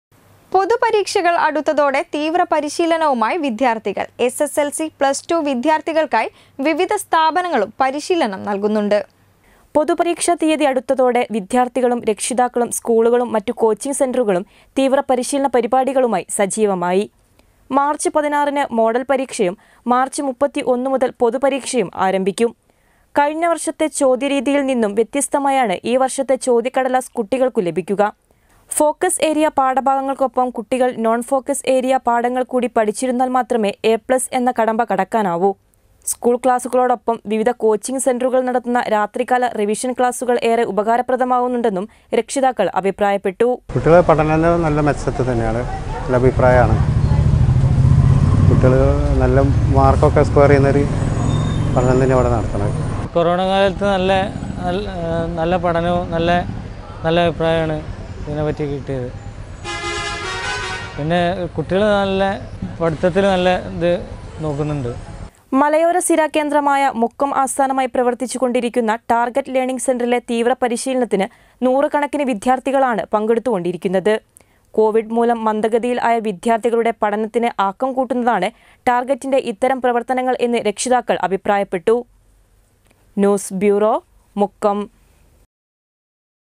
പൊതു പരീക്ഷകൾ അടുത്തതോടെ തീവ്രപരിശീലനവുമായി വിദ്യാർത്ഥികൾ
പൊതു പരീക്ഷകൾ അടുത്തതോടെ തീവ്രപരിശീലനവുമായി വിദ്യാർത്ഥികൾ.എസ്.എസ്.എൽ.സി, പ്ലസ് ടു വിദ്യാർത്ഥികൾക്കായി വിവിധ സ്ഥാപനങ്ങളും പരിശീലനം നൽകുന്നുണ്ട്